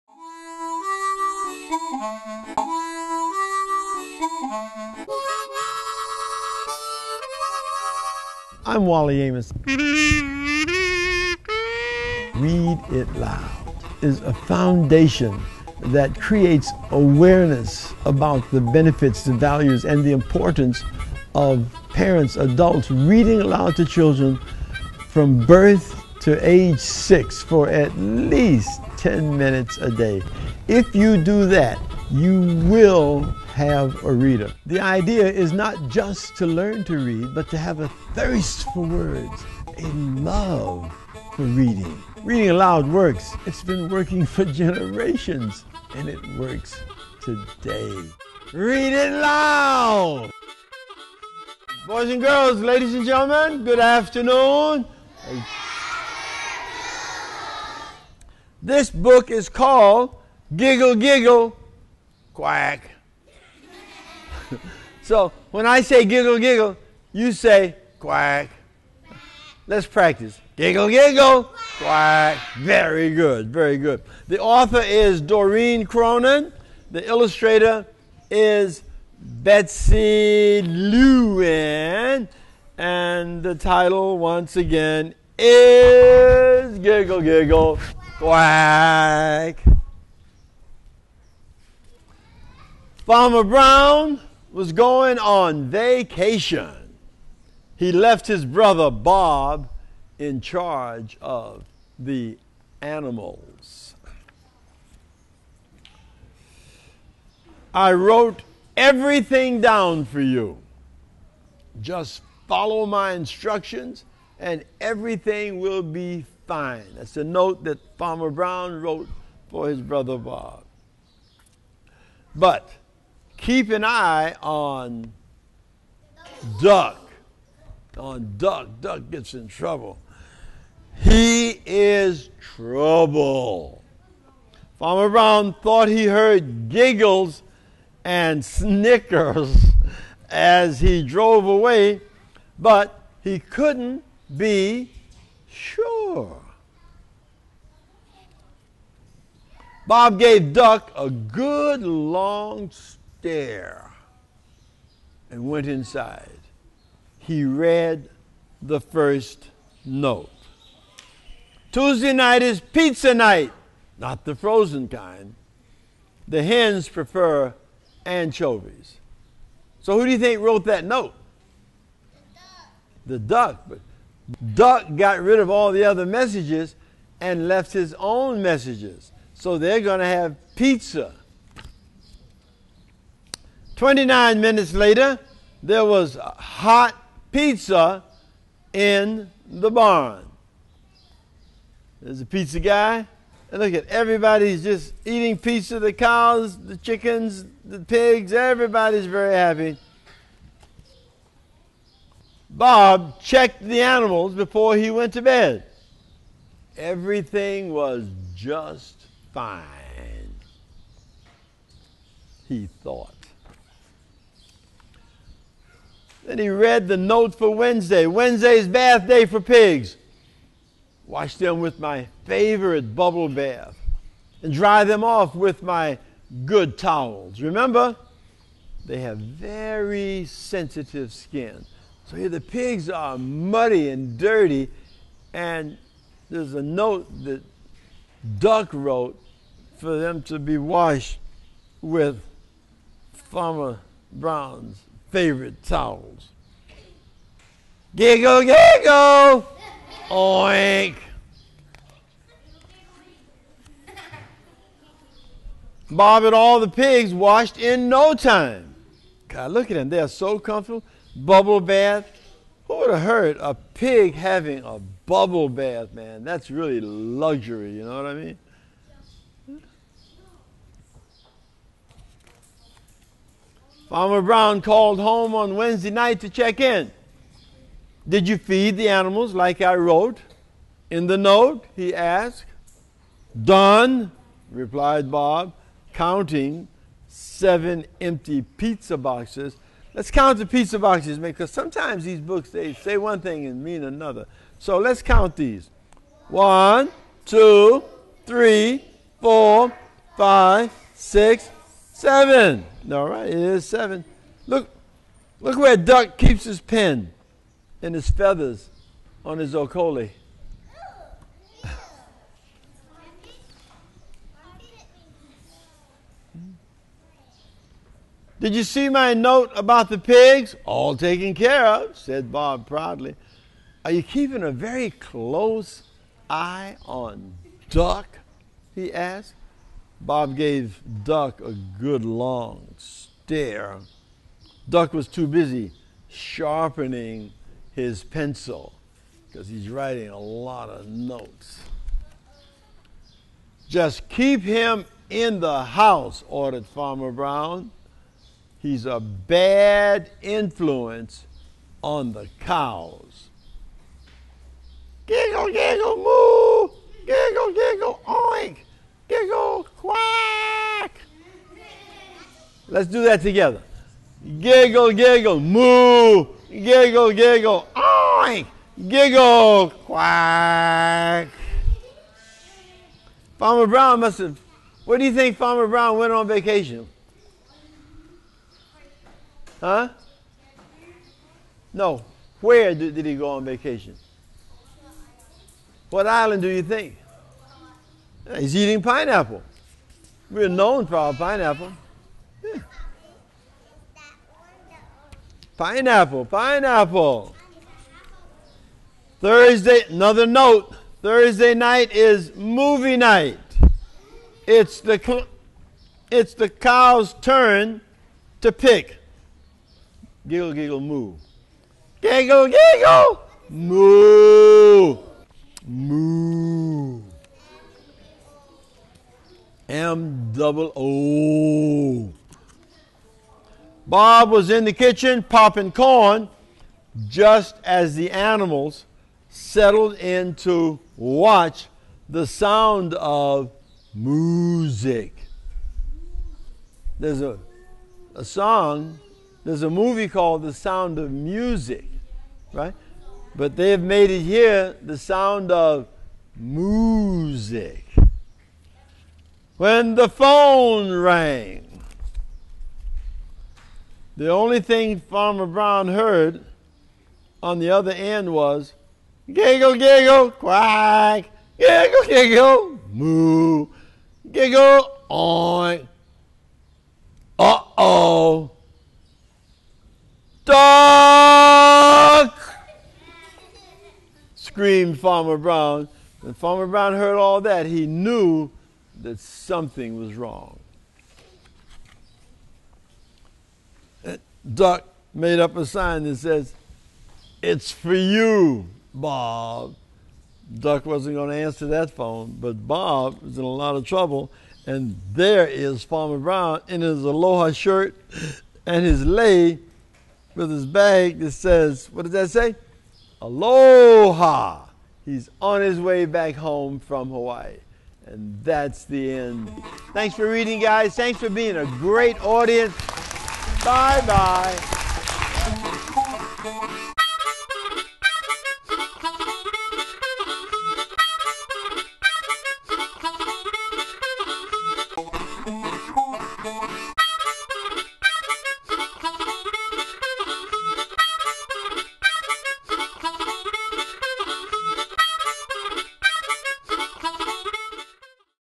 Giggle, Giggle, Quack
Giggle, Giggle, Quack read by Wally Amos.

A mission centering around a simple message.
Take at least 10 minutes at the end of your day to read together.